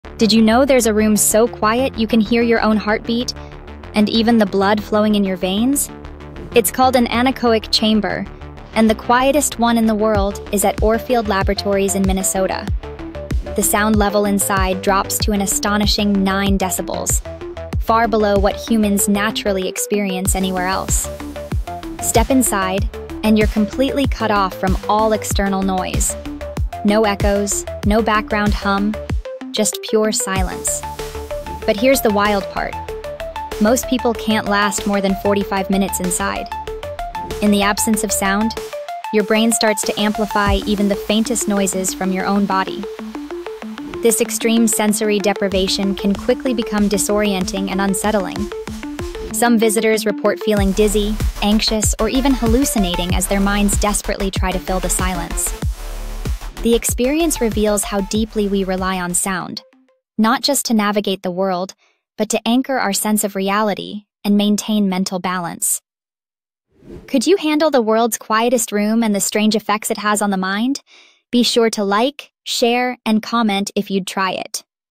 World’s Quietest Room: Silence vs. The Mind
Could you survive total silence? Discover the world's quietest room and its eerie effects on the human mind.